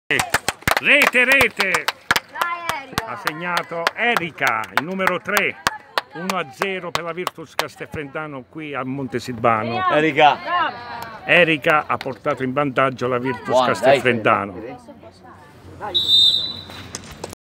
VIRTUS CASTEL FRENTANO CALCIO A5 FEMMINILE COPPA ITALIA
Positivo esordio delle nostre ragazze Domenica scorsa in Coppa Italia a Montesilvano contro la
locale Formazione . Il 1° Tempo era terminato con il risultato parziale di 2 - 1 a nostro favore .Poi nel 2° Tempo ,causa varie assenze di alcune
nostre Atlete , non si sono potuti fare tutti i cambi previsti . Solo due cambi effettuati , mentre gli avversari ne hanno effettuati 7 . Era ovvio che con il grande caldo
durante la Gara ,le nostre ragazze non hanno potuto riprendere fiato , e quindi siamo scesi di ritmo . Comunque una prova superlativa per le nostre Lady che hanno ben figurato contro la blasonata e nobile Società avversaria .
COMPLIMENTI A TUTTE LE RAGAZZE SCESE IN CAMPO ,E UN RINGRAZIAMENTO VA ALLA SQUADRA AVVERSARIA E ALLA SOCIETA' CHE SI E' DIMOSTRATA MOLTO DISPONIBILE ALLE NOSTRE ESIGENZE ,ESSENDO NOI UNA SQUADRA AL DEBUTTO .